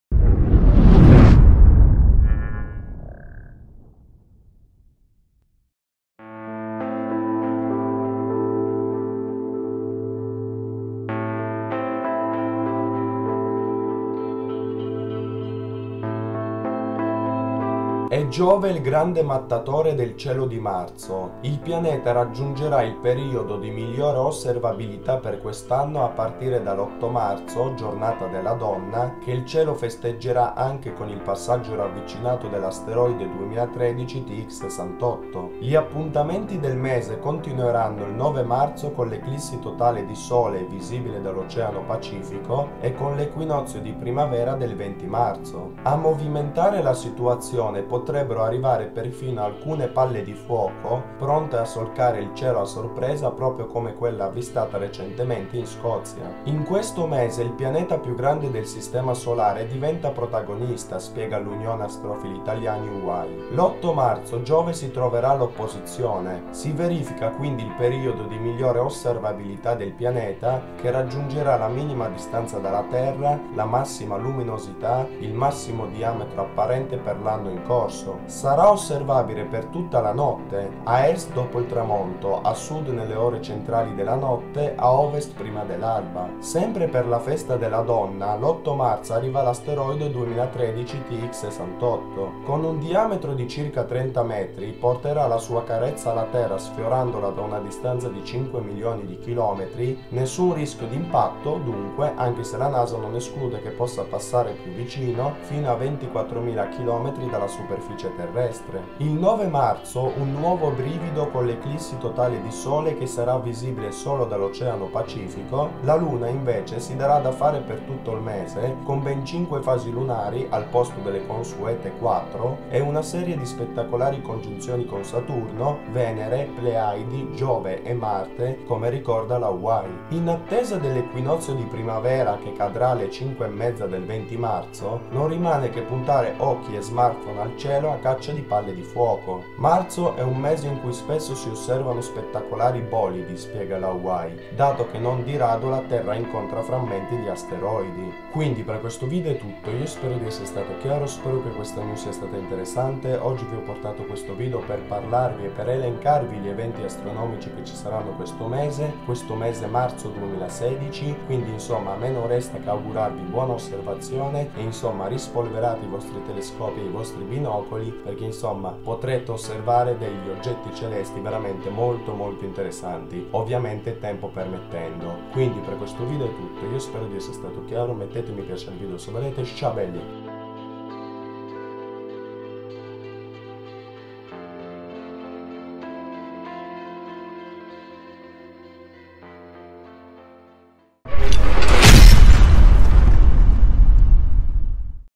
Cielo di marzo 2016: Giove e "palle di fuoco" - Asteroide 2013 TX68, poi l'eclissi di Sole
--Ehi, tu! Dai un'occhiata qui!--
In questo video vi parlerò degli eventi astronomici che ci aspettano questo mese di marzo di quest'anno 2016.
Li eventi sono tanti e gli oggetti celesti da osservare non mancano sicuramente quindi non mi resta che augurarvi buona osservazione e sopratutto cieli sereni perchè per chi vorrà osservare qualcosa, ne avrà sicuramente bisogno!
Spero che il video vi piaccia & vi sia utile!!!

Canali di divulgazione scientifica astronomica
Astronomical Science Channel

Social Network (Follow me on:)